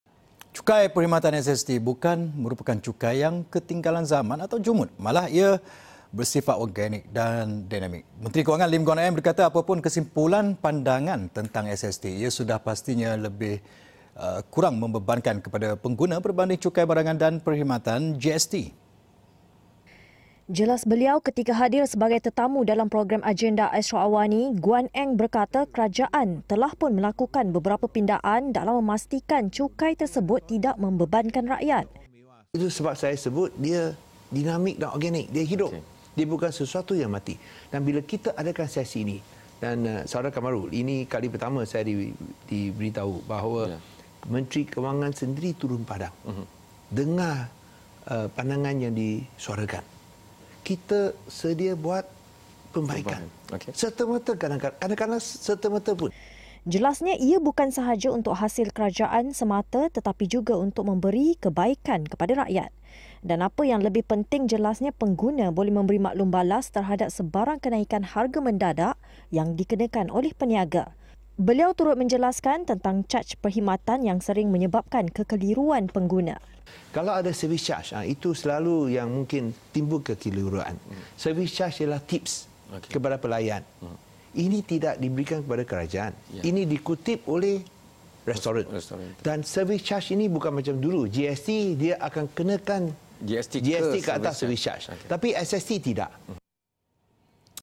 SST merupakan cukai, organik dan dinamik
Cukai perkhidmatan SST bukan merupakan cukai yang ketinggalan zaman atau jumud malah ia bersifat organik dan dinamik.